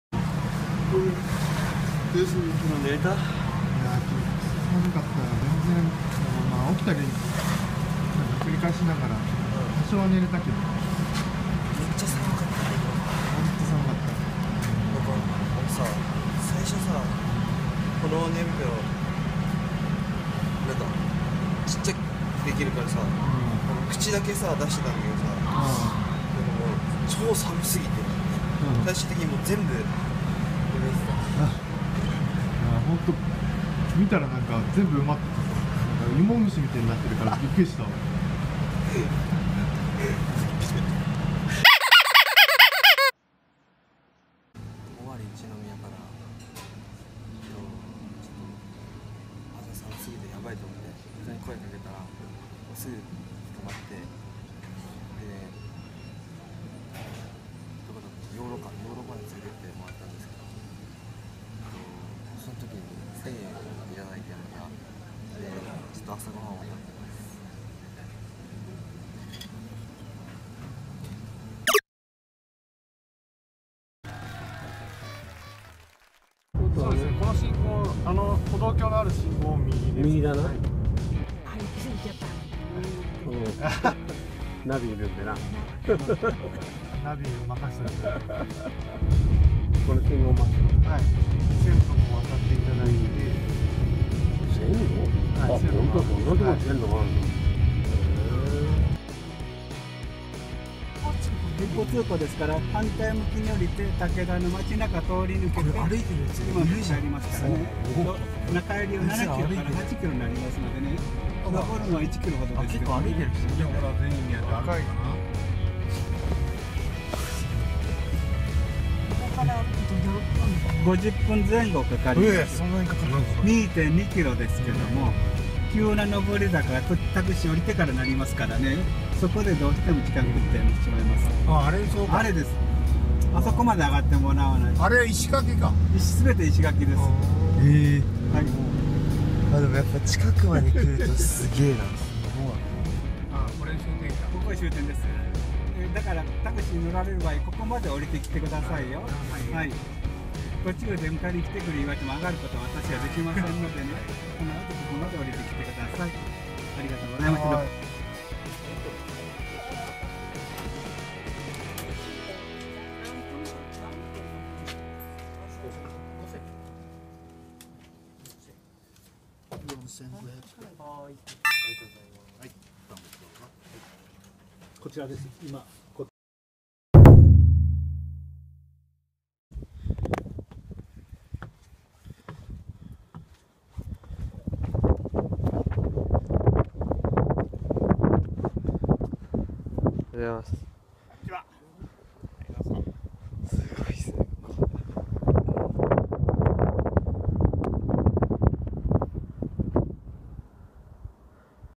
ヒッチハイクで探る兵庫県竹田城の真実 「天空の城」と呼ばれた城の謎に迫る
５千円ヒッチハイク #4